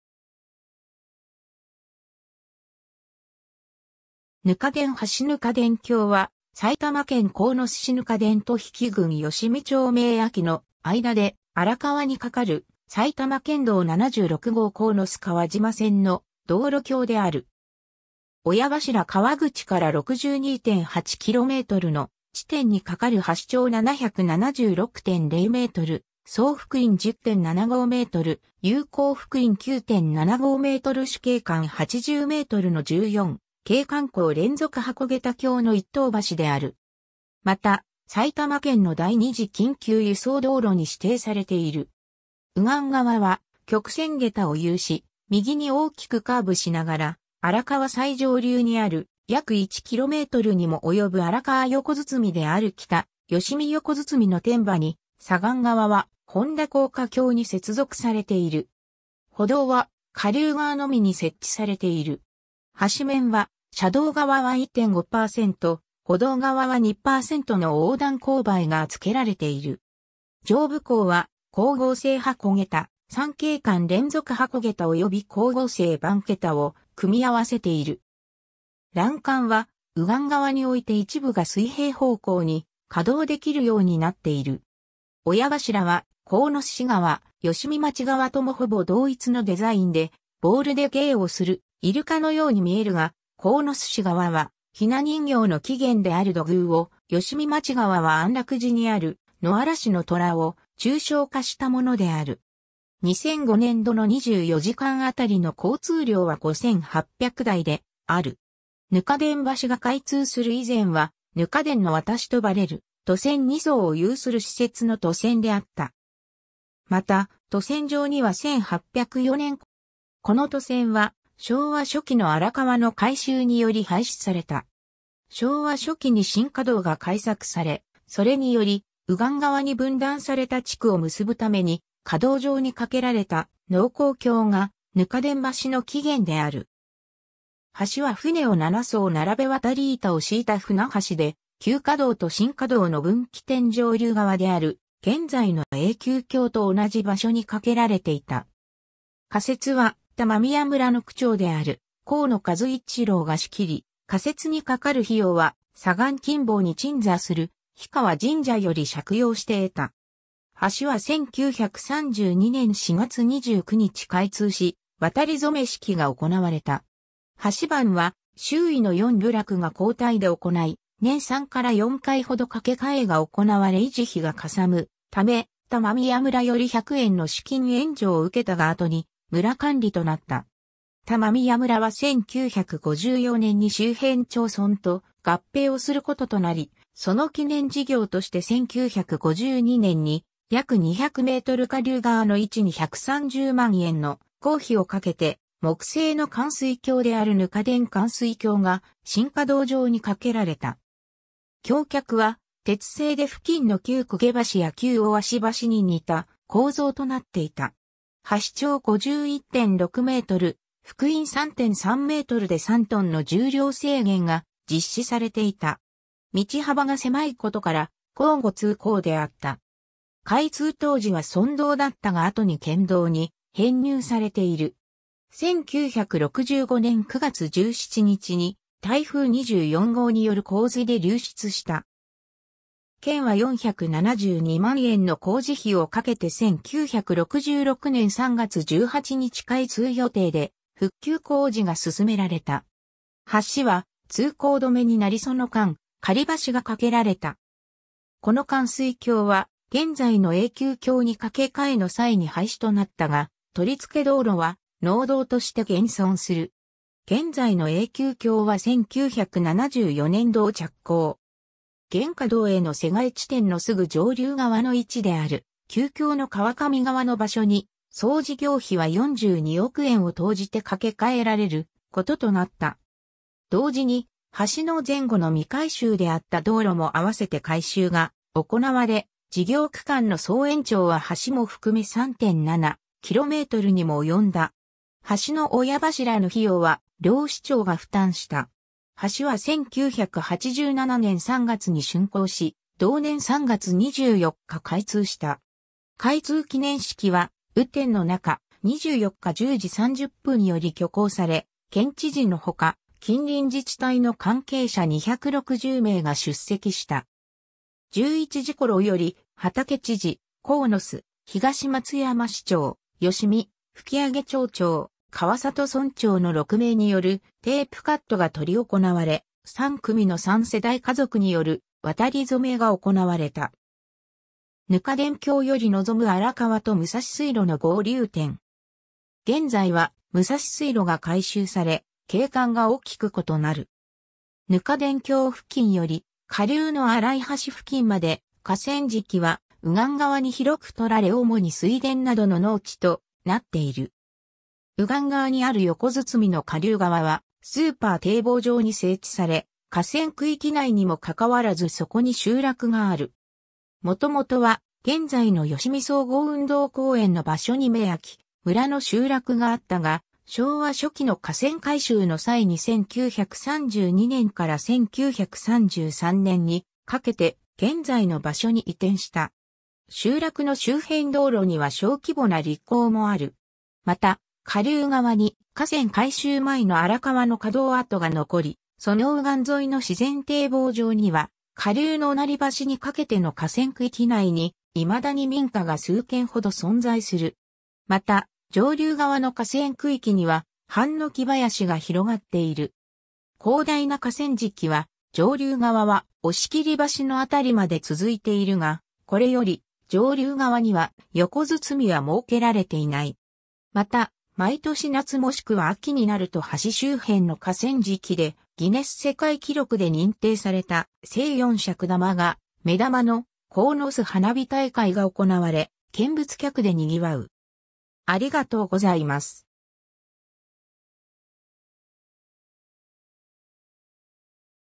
糠田橋
糠田橋（2012年10月） 糠田橋（ぬかたばし）は、埼玉県鴻巣市糠田と比企郡吉見町明秋の間で荒川に架かる埼玉県道76号鴻巣川島線の道路橋である。
 親柱（鴻巣市側） 河口から62.8 kmの地点（右岸側。左岸側は63.2 km）に架かる橋長776.0メートル、総幅員10.75メートル、有効幅員9.75メートル主径間80メートルの14径間鋼連続箱桁橋の1等橋（TL-20）である。
また、埼玉県の第二次緊急輸送道路に指定されている。
右岸側は曲線桁を有し、右に大きくカーブしながら荒川最上流にある約1 kmにも及ぶ荒川横堤である北吉見横堤の天端に、左岸側は本田高架橋に接続されている。
歩道は下流側のみに設置されている。
橋面は、車道側は1.5パーセント、歩道側は2パーセントの横断勾配がつけられている。
上部工は鋼合成箱桁、3径間連続箱桁および鋼合成鈑桁を組み合わせている。
欄干は右岸側において一部が水平方向に可動できる様になっている。
親柱は鴻巣市側、吉見町側共ほぼ同一のデザインで、ボールで芸をするイルカのように見えるが、鴻巣市側は雛人形の起源である土偶を、吉見町側は安楽寺（吉見観音）にある「野荒しの虎」を抽象化したものである。
2005年（平成17年）度の24時間当たりの交通量は5,800台である。
 糠田橋が開通する以前は、糠田の渡しとばれる渡船2艘を有する私設の渡船であった。
また、渡船場には1804年（文化元年）頃までに設立された糠田河岸場が併設されていた。
この渡船は昭和初期の荒川の改修により廃止された。
 昭和初期に新河道が開削され、それにより右岸側に分断された地区を結ぶために河道上に架けられた農耕橋が糠田橋の起源である。
橋は船を7艘並べ渡り板を敷いた舟橋で、旧河道と新河道の分岐点上流側である現在の永久橋と同じ場所に架けられていた。
架設は田間宮村の区長である河野和一郎が仕切り、架設に掛かる費用は左岸近傍に鎮座する氷川神社より借用して得た。
橋は1932年（昭和7年）4月29日開通し、渡り初め式が行われた。
橋番は周囲の4部落が交代で行ない、年3-4回ほど架け替えが行なわれ維持費がかさむため、田間宮村より100円の資金援助を受けたが後に村管理となった。
 田間宮村は1954年（昭和29年）に周辺町村と合併をすることとなり、その記念事業として1952年（昭和27年）に約200メートル下流側の位置に130万円の工費を掛けて木製の冠水橋である糠田冠水橋が新河道上に架けられた。
橋脚は鉄製で付近の旧久下橋や旧大芦橋に似た構造となっていた。
橋長51.6メートル、幅員3.3メートルで3トンの重量制限が実施されていた。
道幅が狭いことから交互通行であった。
開通当時は村道だったが後に県道に編入されている。
 1965年9月17日に台風24号による洪水で流失した。
県は472万円の工事費を掛けて1966年（昭和41年）3月18日開通予定で復旧工事が進められた。
橋は通行止めになりその間、仮橋が架けられた。
 この冠水橋は現在の永久橋に架け替えの際に廃止となったが、取り付け道路は農道として現存する。
 現在の永久橋は1974年（昭和49年）度着工。
現河道への瀬替え地点のすぐ上流側の位置である旧橋の川上側の場所に、総事業費は42億円を投じて架け替えられることとなった。
同時に橋の前後の未改修であった道路も合わせて改修が行われ、事業区間の総延長は橋も含め3.7 kmにも及んだ。
橋の親柱の費用は両市町が負担した。
橋は1987年（昭和62年）3月に竣工し、同年3月24日開通した。
開通記念式は雨天の中、24日10時30分より挙行され、県知事のほか、近隣自治体の関係者260名が出席した。
11時頃より、畑知事、鴻巣・東松山市長、吉見・吹上町長、川里村長の6名によるテープカットが執り行われ、三組の三世代家族による渡り初めが行なわれた。
 糠田橋より望む荒川と武蔵水路の合流点。
現在は武蔵水路が改修され、景観が大きく異なる。
 糠田橋付近より下流の荒井橋付近まで、河川敷は右岸側に広くとられ主に水田などの農地となっている。
 右岸側にある横堤の下流側はスーパー堤防状に整地され、河川区域内にもかかわらずそこに集落がある。
元々は現在の吉見総合運動公園の場所に明秋村の集落があったが、昭和初期の河川改修の際に1932年（昭和7年）から1933年（昭和8年）にかけて現在の場所に移転した。
集落の周辺道路には小規模な陸...